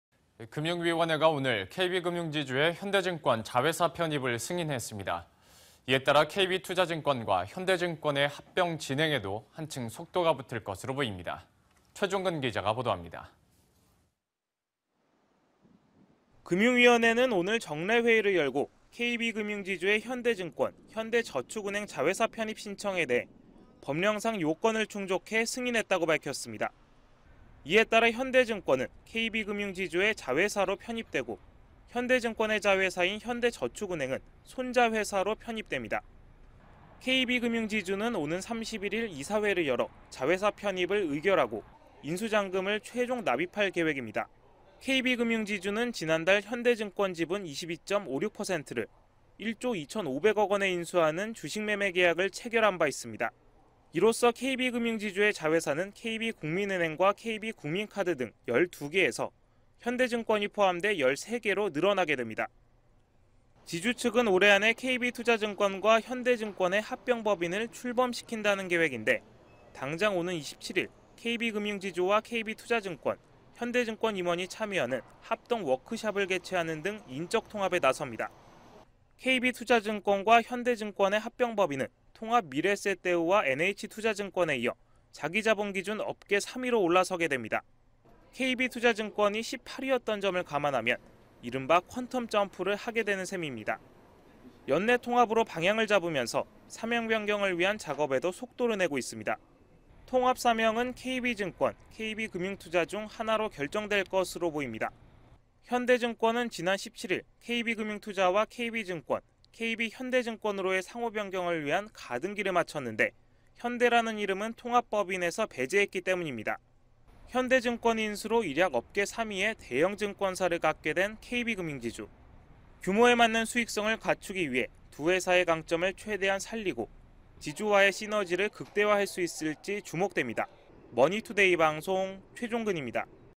KB금융, 현대증권 인수 '최종 승인'..18→3위로 퀀텀 점프
05/25 MTN 투데이

금융위원회가 오늘(25일) KB금융지주의 현대증권 자회사 편입을 승인했습니다. 이에 따라 KB투자증권과 현대증권의 합병 진행에도 한층 속도가 붙을 것으로 보입니다. 최종근 기자가 보도합니다.